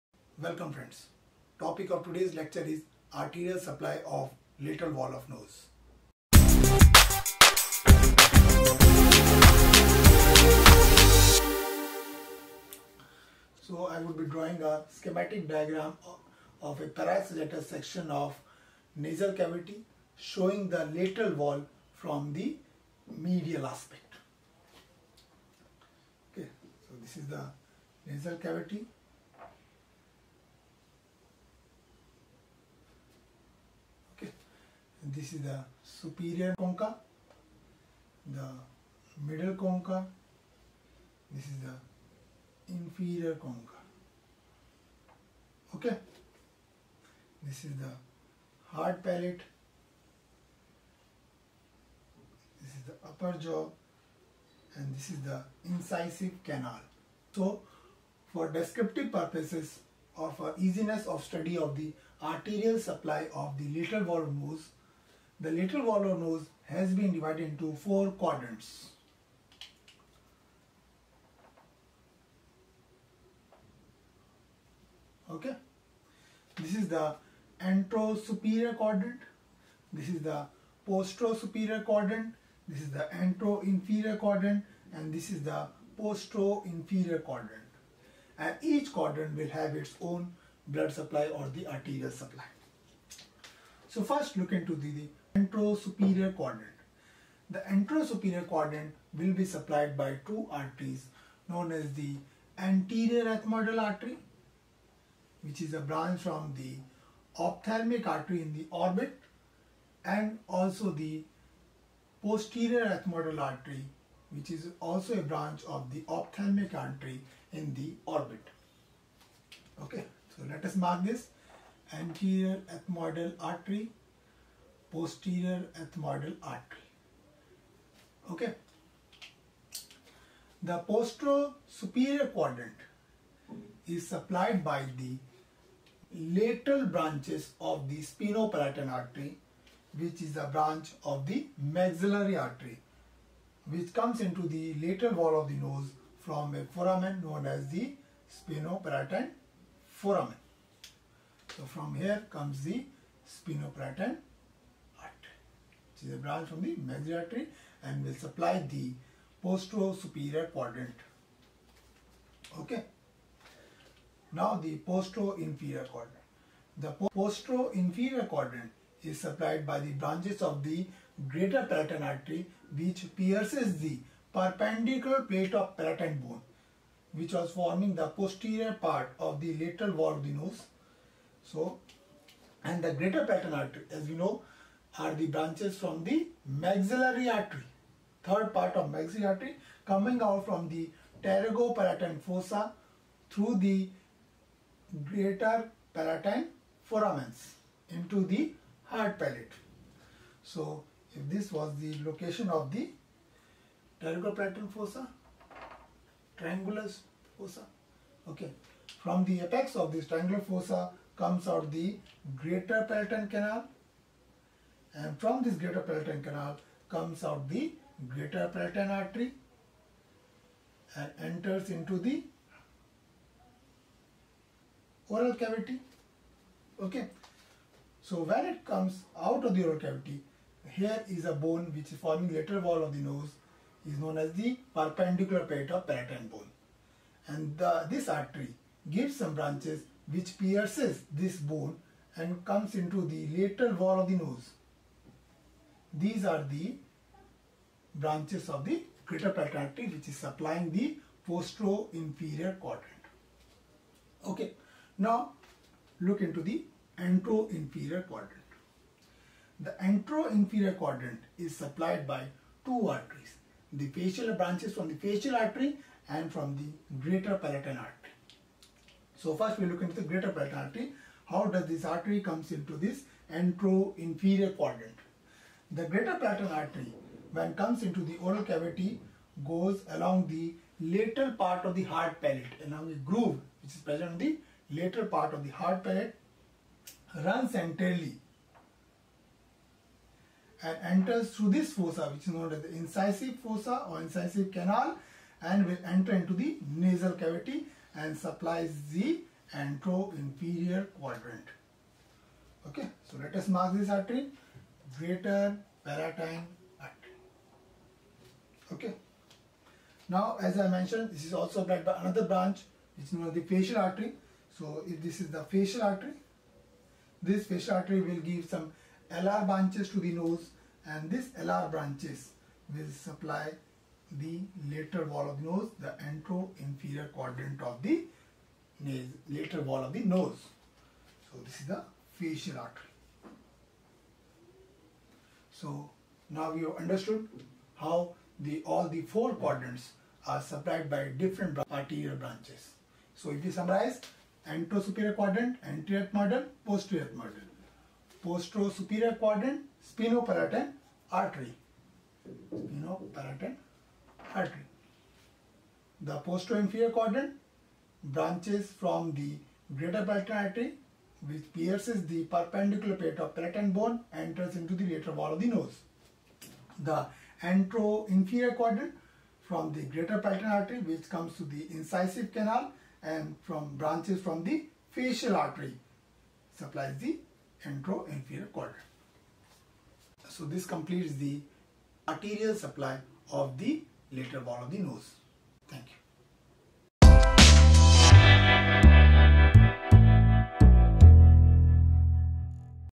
Arterial supply of Lateral wall of Nasal cavity
Arterial supply of Lateral wall of Nasal cavity described in detail.

About : Anatomy Classes By Dr. Sushant is a YouTube Channel, where you will find anatomy videos in english :)